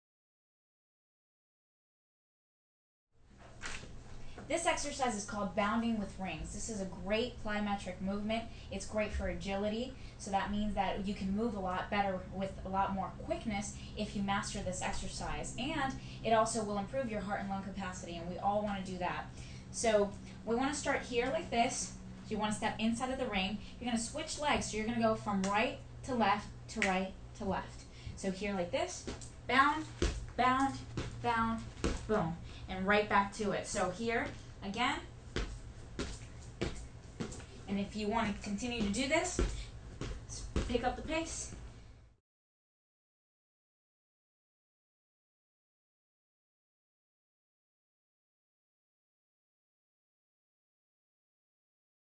PACE Fitness Exercise - Bounding with Rings - Plus FREE PACE eBook
Certified PACE Instructor, Coach Yari demonstrates Bounding with Rings.

Find out why PACE Express - The 12-Minute Fitness Revolution is so much better than the fitness programs the "cardio experts" recommend.